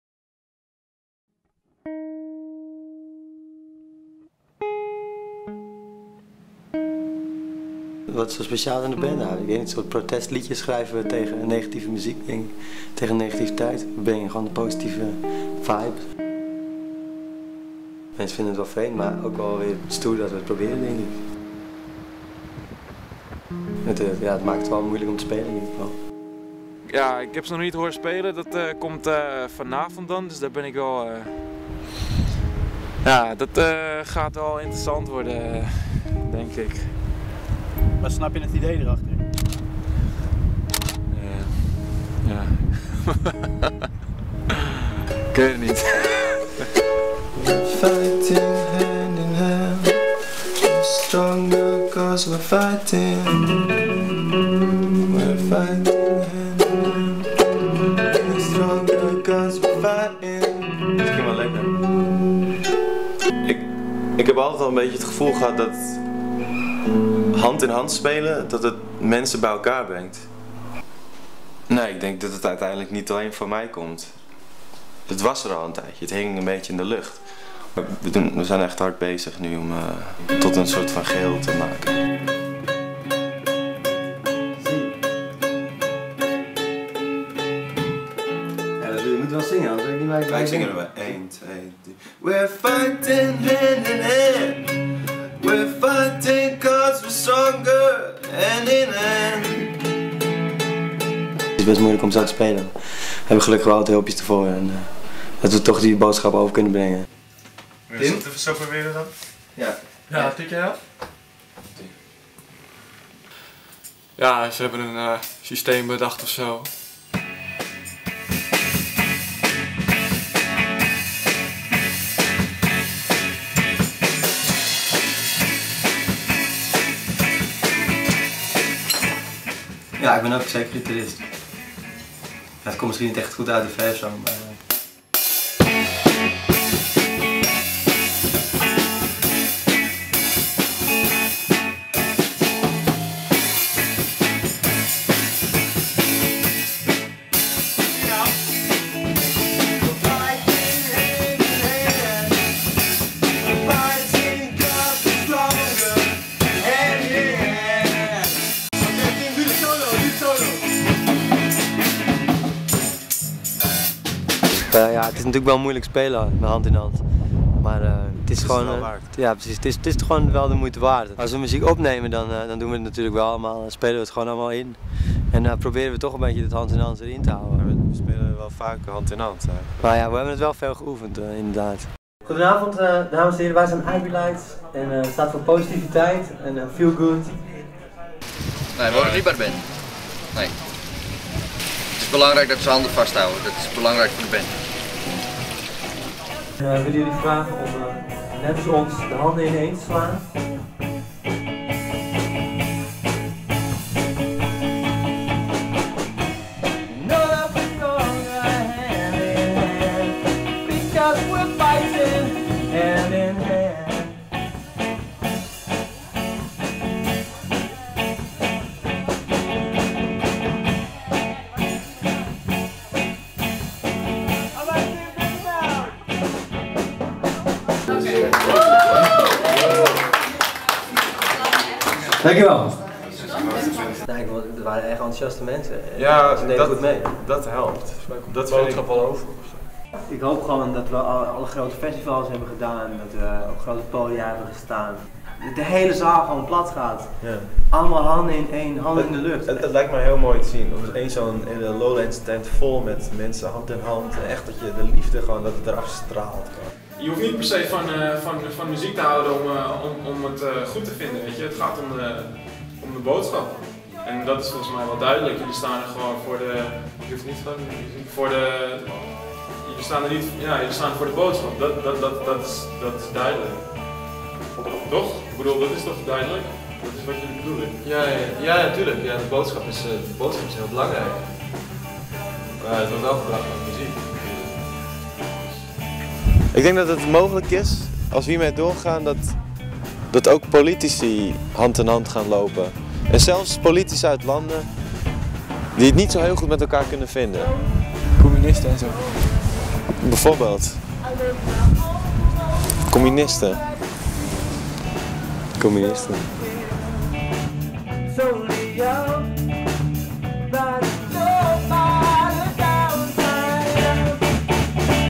Performing Positivity (Irielites)
A short rockumentary about the band 'Irielites', a band working to fight negativity and bring positivity through their way of playing.

Irielites consists of Bob Maars and Niels Visser. The film is directed by Emil Pabon with the help of Rick Bruins.